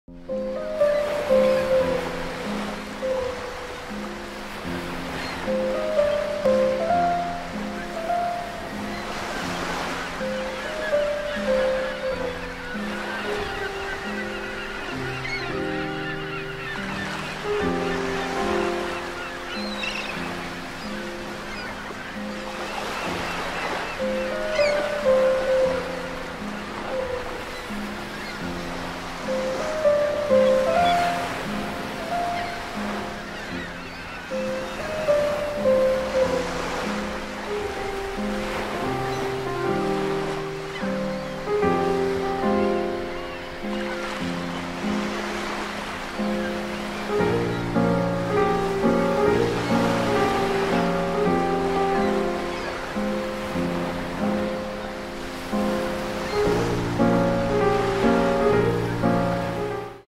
The Seagull Master. Trieste, Italy. (2016)
Just one man feeding seagulls at the pier. Awesome performance! Shot on RED. The pier of Trieste, Italy.